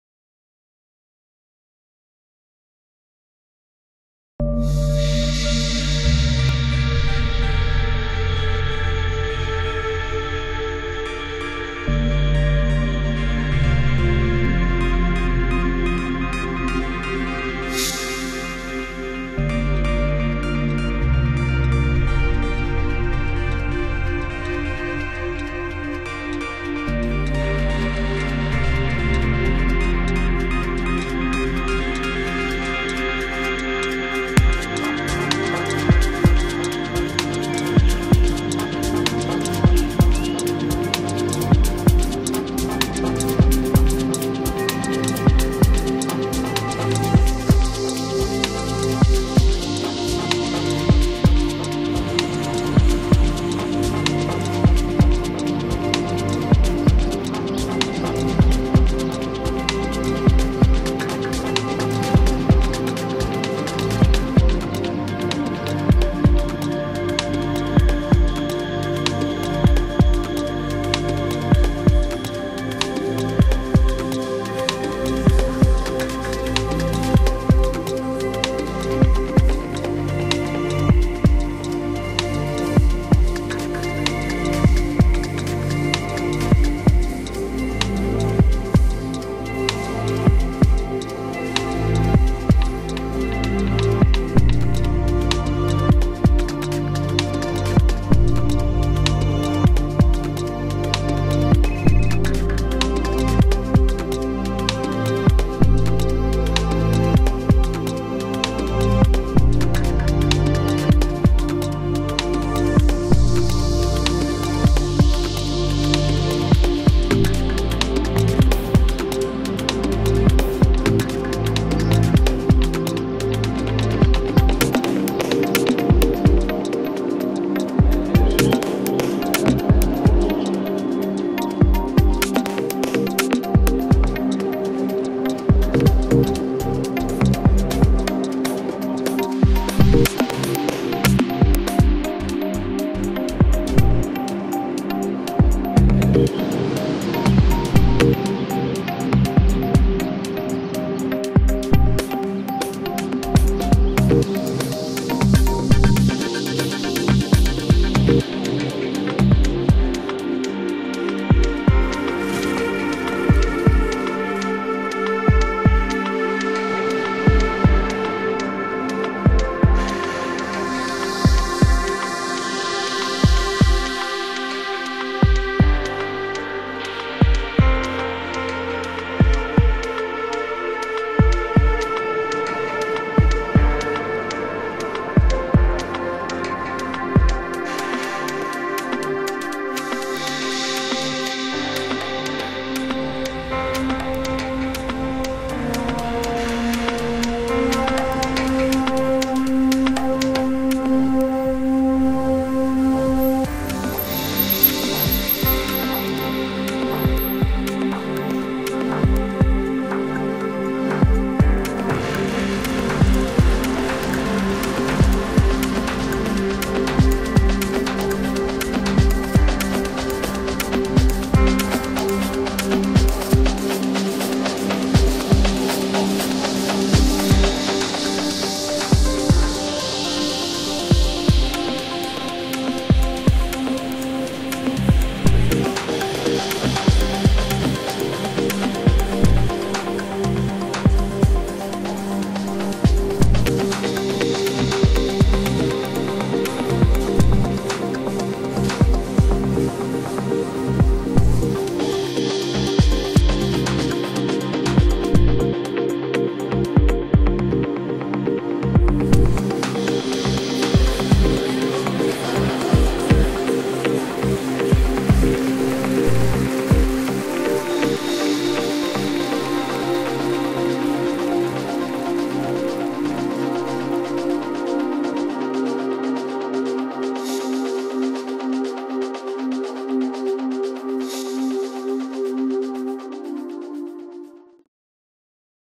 Doncaster's Civic and Cultural Quarter movie
This movie showcases Doncaster's planned Civic and Cultural Quarter. The film gives you a glimpse of what you can expect from the new performance venue, public square and other related developments which are designed to transform Waterdale and the town centre. Real life people are shown experiencing the new facilities.